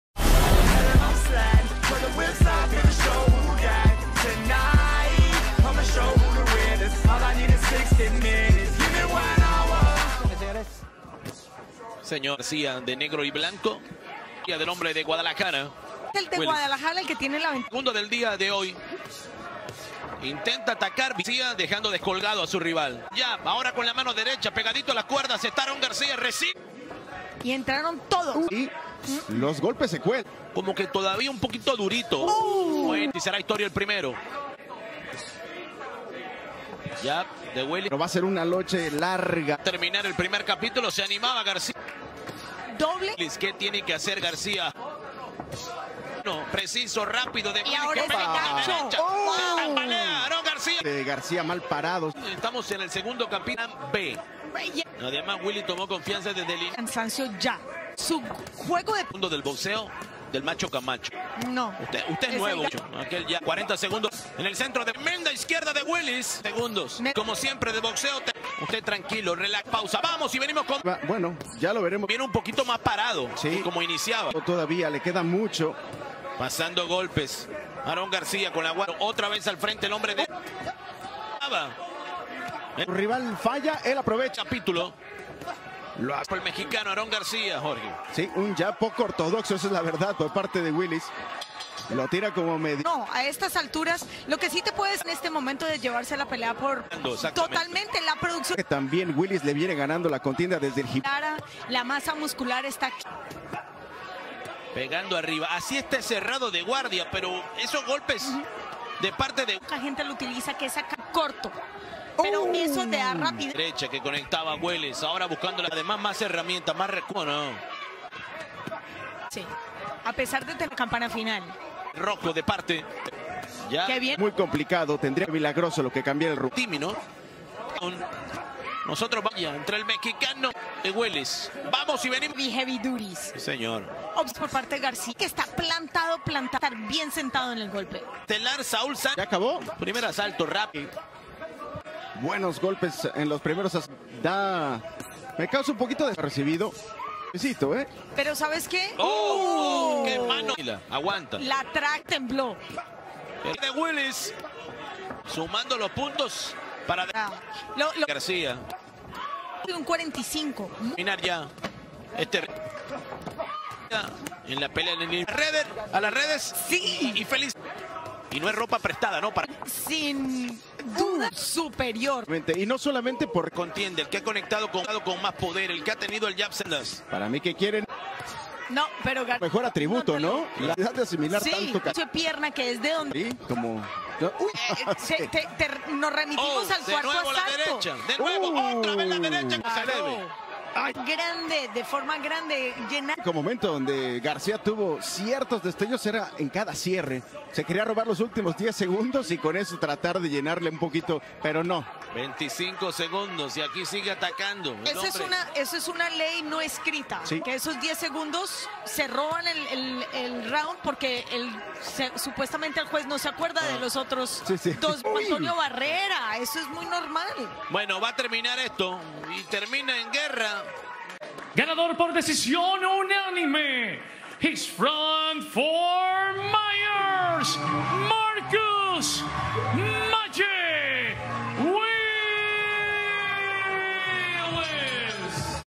Marcus Willis vs Aaron Garcia highlights
Highlights of Magic Marcus Willis' unanimous decision victory over Aaron Garcia on Telemundo 03/19/21 in Tampa, Fl